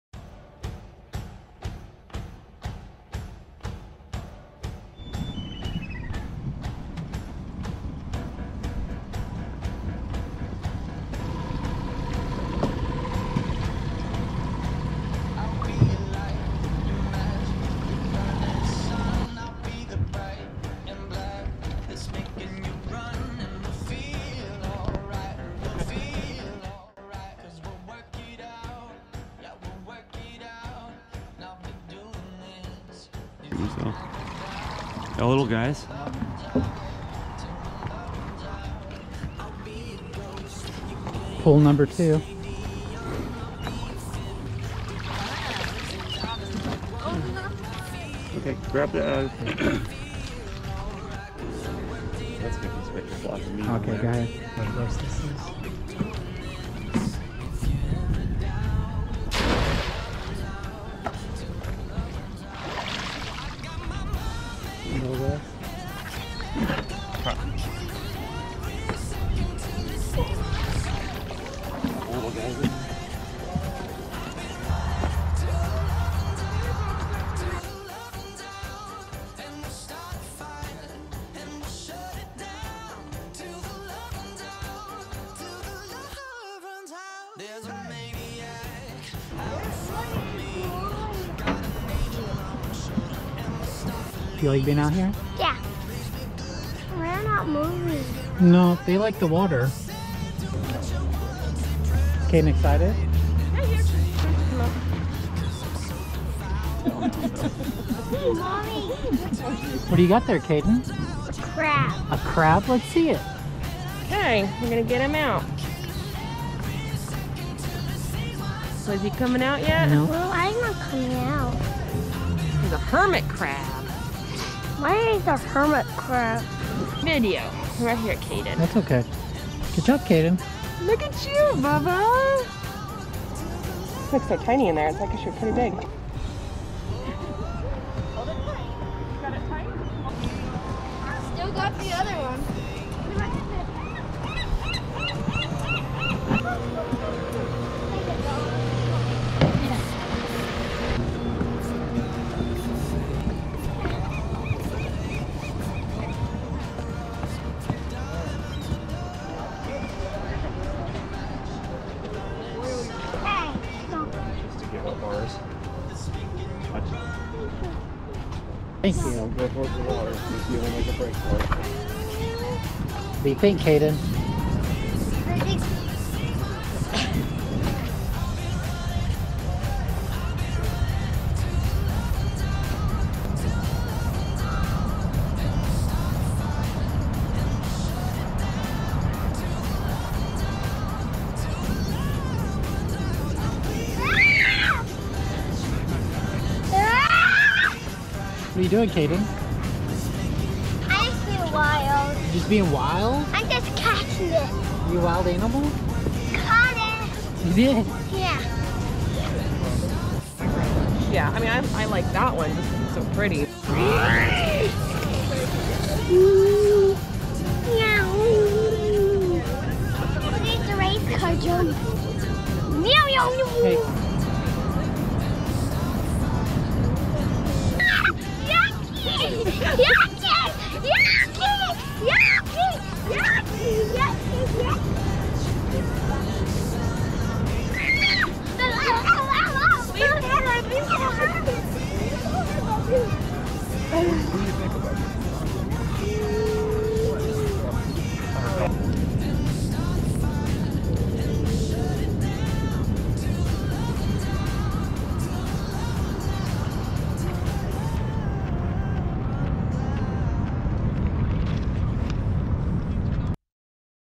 Crabbing at Nehalem Bay, Oregon
Kaden goes crabbing in Nehalem Bay, Oregon with his aunt and uncle.